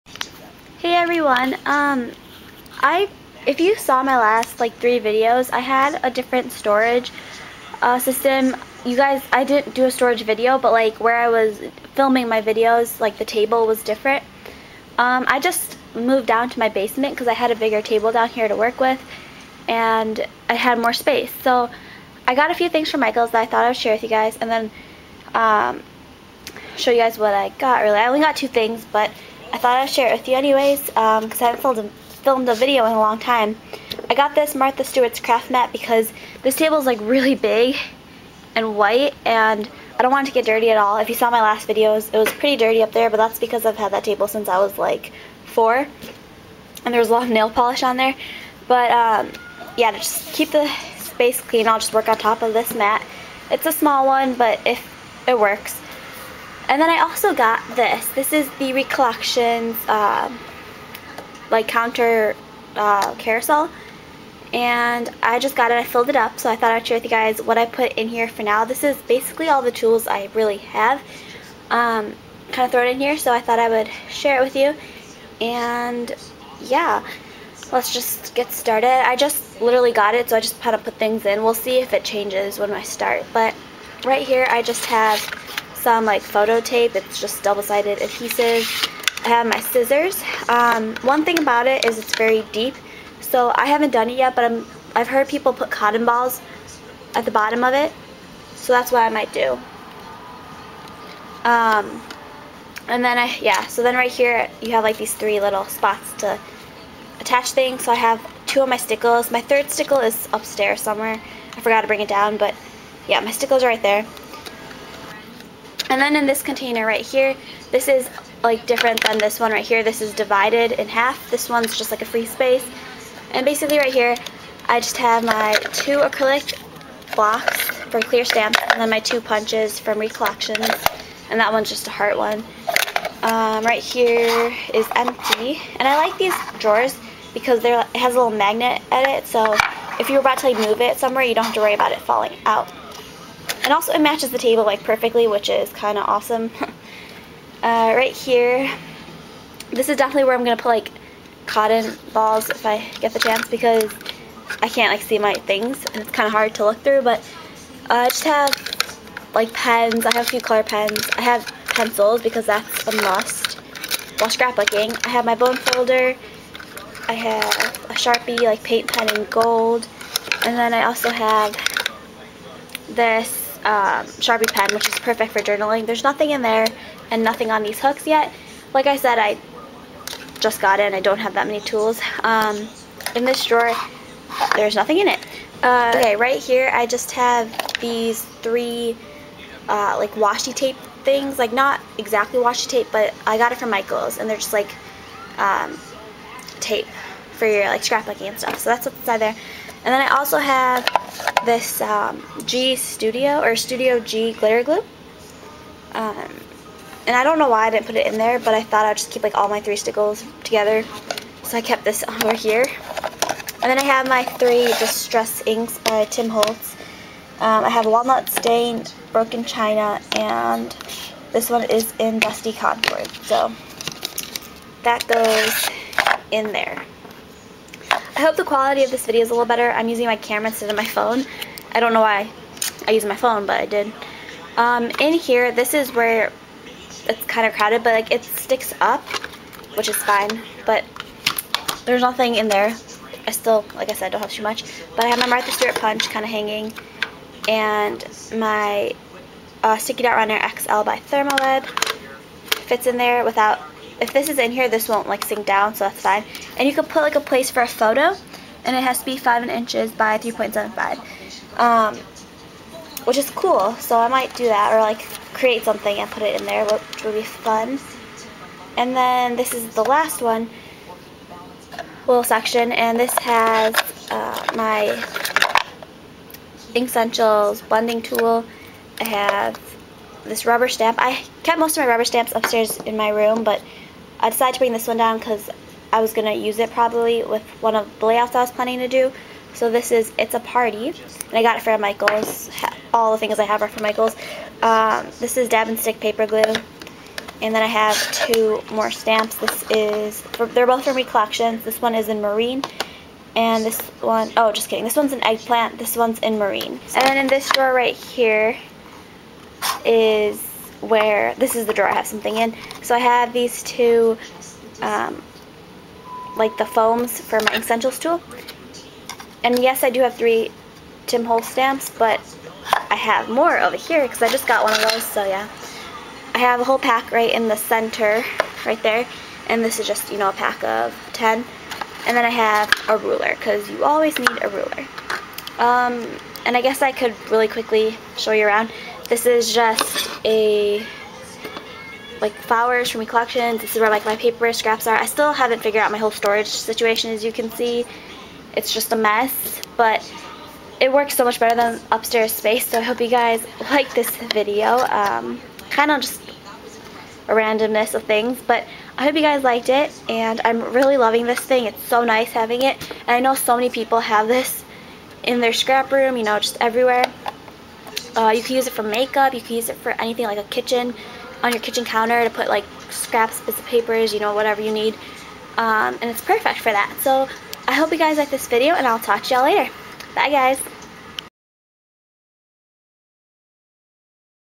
Whats inside my new Recollections Desktop Carousel??!!
Hey everyone, I haven't made a video in a long time. Since I changed the place where I will be doing projects from now on, I thought I would show a little preview of where the new place was. I also did a tour of the Recollections Desktop Carousel that I bought from Michaels. I hope you enjoy!! :)